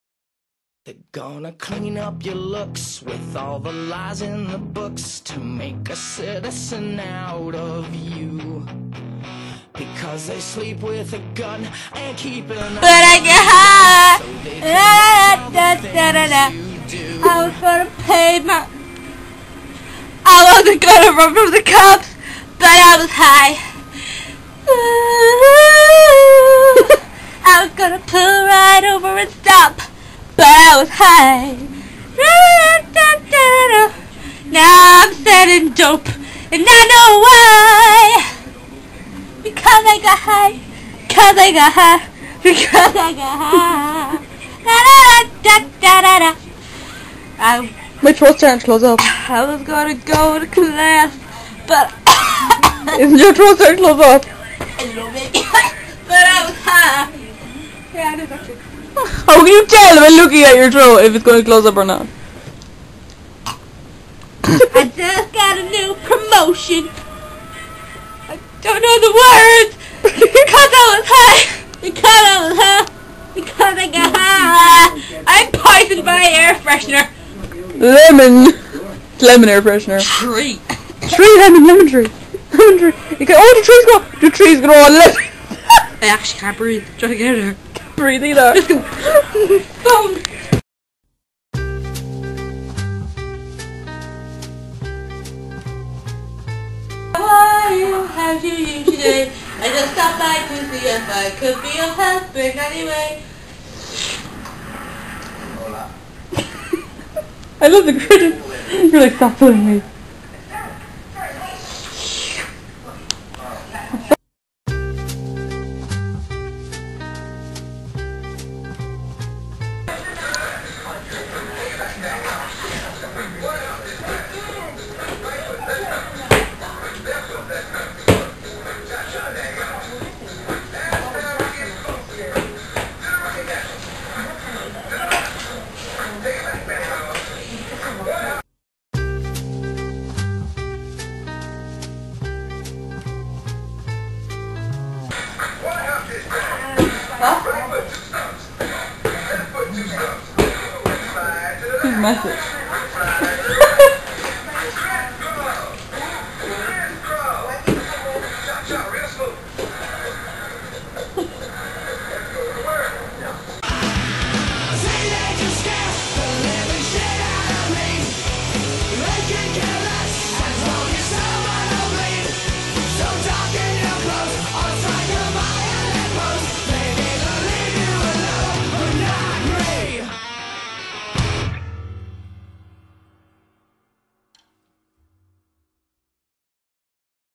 Poisoned By Air Freshener
this is at a random sleepover when tammy tried to kill both of us by emptying a can of air freshener in her room..enjoy?